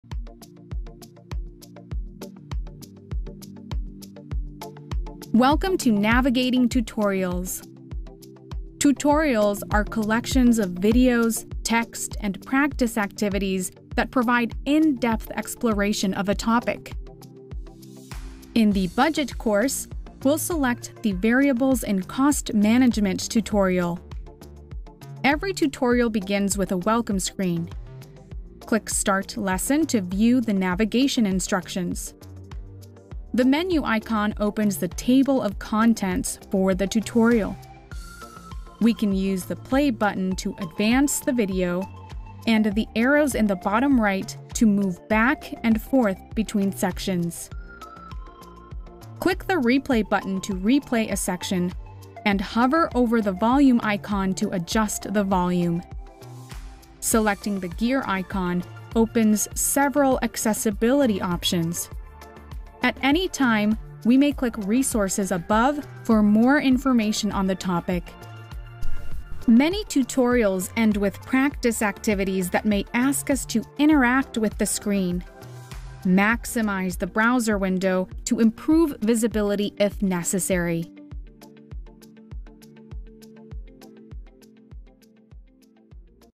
Navigating Autodesk Tutorials | Autodesk Construction Cloud Learning Center
Need more information about the course you’re working through? The tutorial tools provide users with a collection of videos, text, and practice activities to help you better understand the associated content. Get started with your first Learning Center course today.